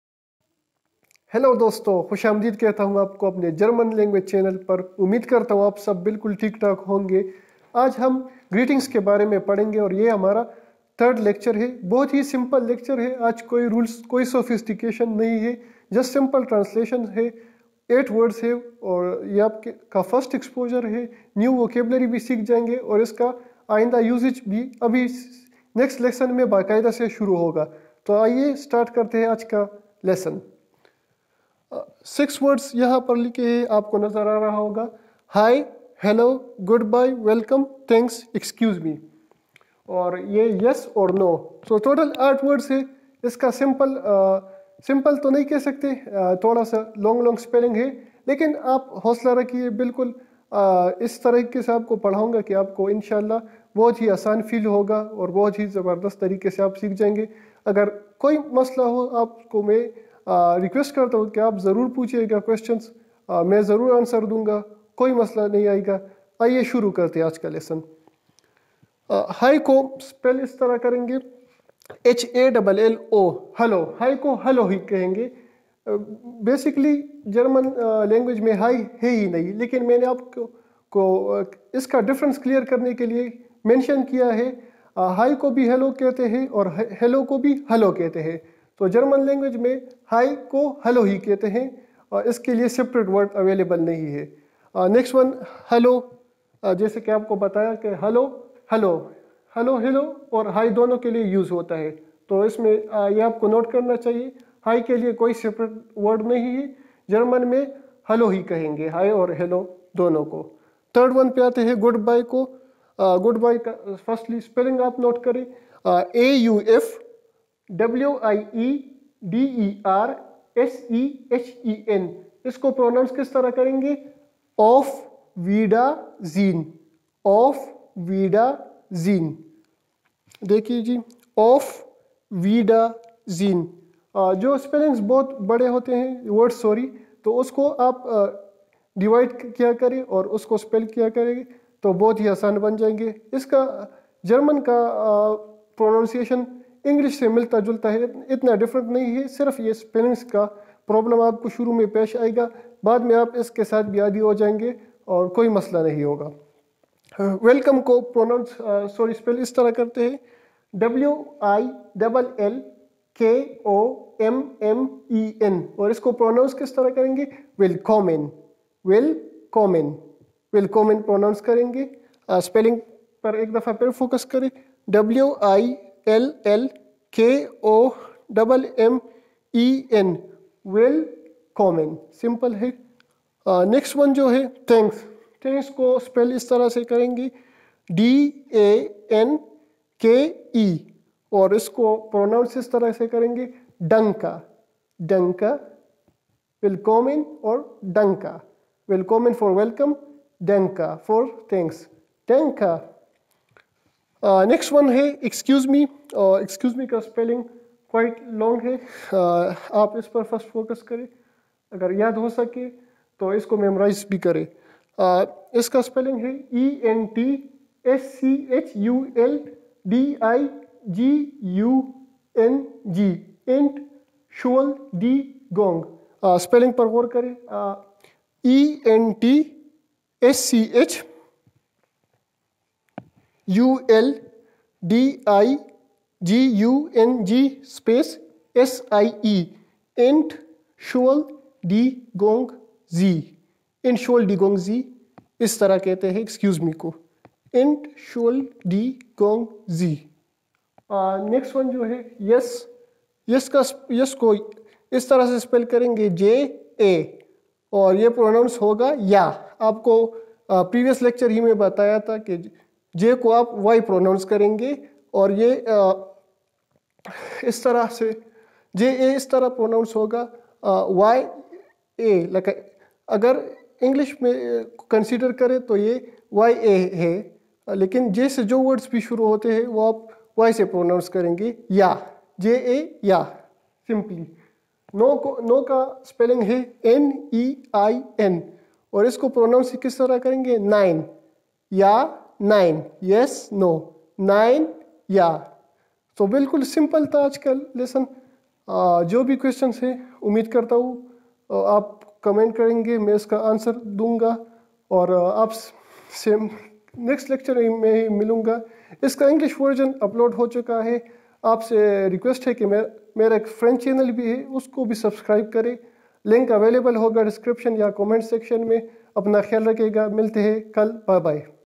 German for beginners | Course Level 1/8 | 3rd lesson-"Greetings" | Hindi/ Urdu
Hi everyone! Umeed karta Hoon Aap sab theek honge! Ye Mera 3rd lesson hai iska English version upload hochuka hai! Mere French channel k sath yahan connect hosakte Hain Aap